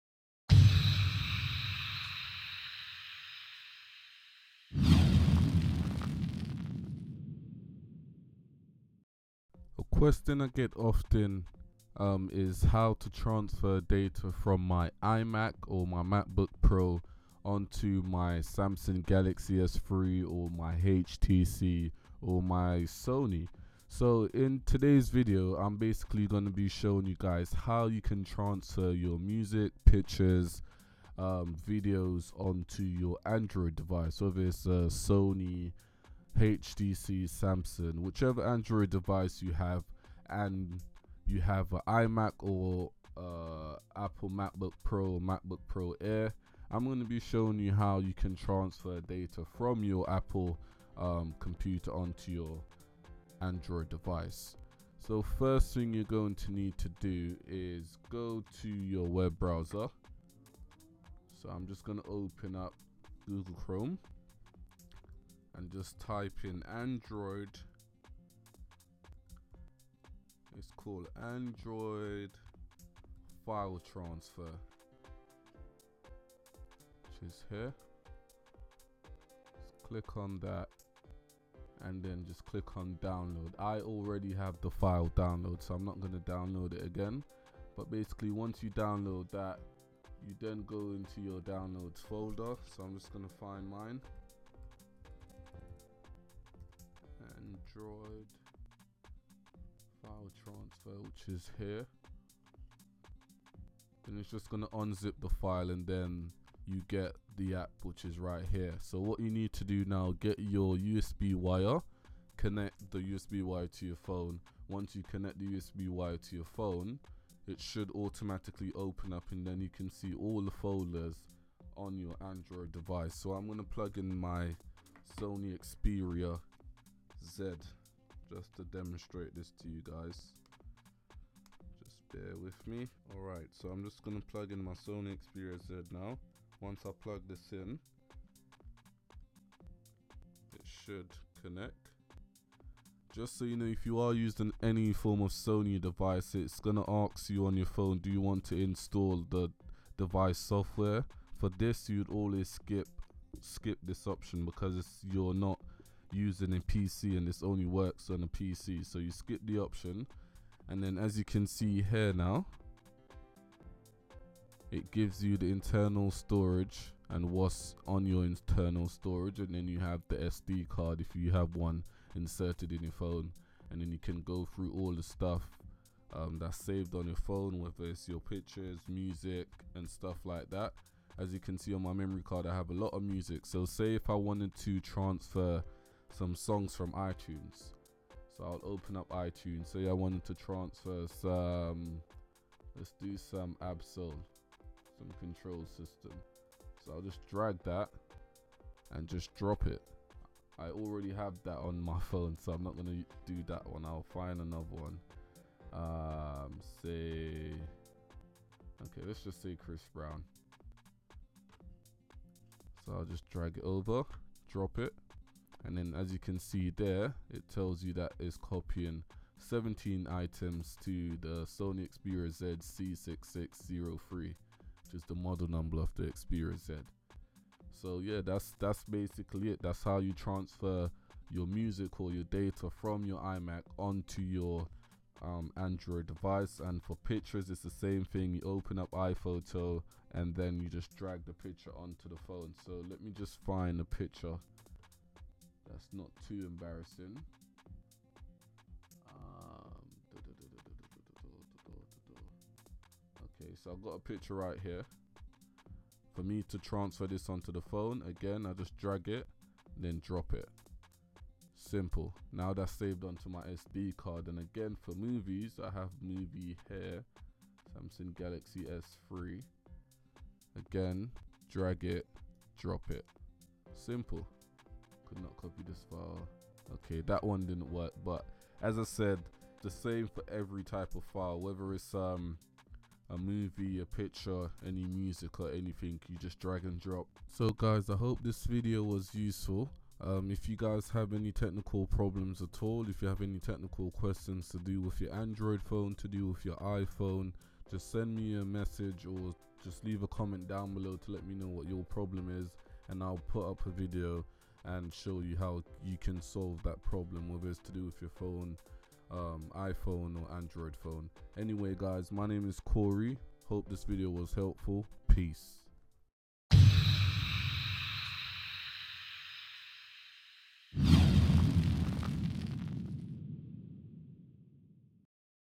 ANDROID "Tips & Tricks" ( data transfer from Apple computer's)
Hey, thinking about buying an android device but not sure if you can transfer data from data from your apple computer? Check out this video where i demo how you can do this with any apple computer. if